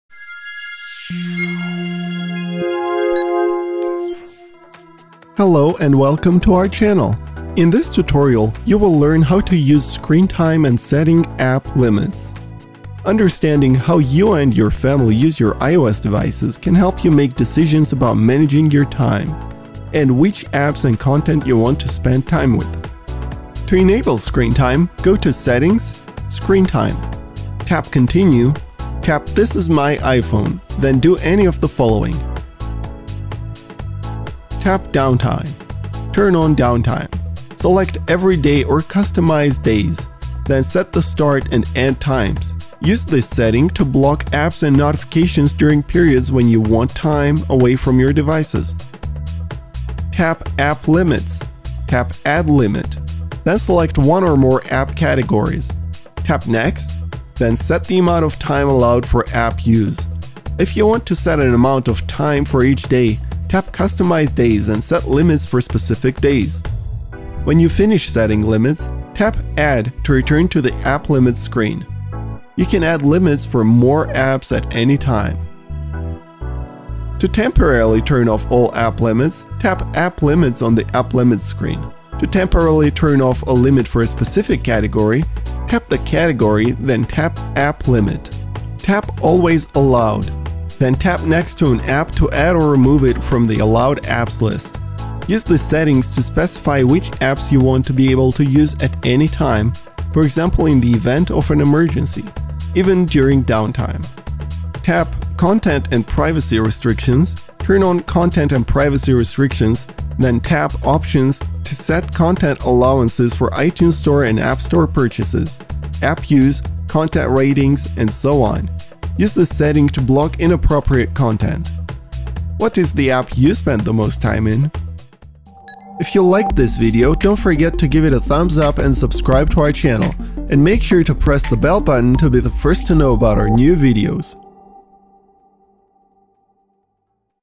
iOS 13: How to Use Screen Time and Setting App Limits? Communication Limits & Parental Control
In this tutorial you will learn how to Use Screen Time and Setting App Limits.

Understanding how you and your family use your iOS devices can help you make decisions about managing your time, and which apps and content you want to spend time with.

To enable Screentime:

➡️ Go to Settings  - Screen Time.
➡️ Tap Continue, tap This is My iPhone, then do any of the following:

➕ Tap Downtime, turn on Downtime, select Every Day or Customize Days, then set the start and end times. Use this setting to block apps and notifications during periods when you want time away from your devices.

➕ Tap App Limits, tap Add Limit, then select one or more app categories. Tap Next, then set the amount of time allowed for app use. (If you want to set an amount of time for each day, tap Customize Days and set limits for specific days.) When you finish setting limits, tap Add to return to the App Limits screen. You can add limits for more apps at any time.

➕ To temporarily turn off all app limits, tap App Limits on the App Limits screen. To temporarily turn off a limit for a specific category, tap the category, then tap App Limit.

➕ Tap Always Allowed, then tap next to an app to add or remove it from the Allowed Apps list. Use this setting to specify which apps you want to be able to use at any time (for example, in the event of an emergency), even during downtime.

➕ Tap Content & Privacy Restrictions, turn on Content & Privacy Restrictions, then tap options to set content allowances for iTunes Store and App Store purchases, app use, content ratings, and so on. Use this setting to block inappropriate content.

❓💬 What is the app you spend the most time in? Please let us know in comments.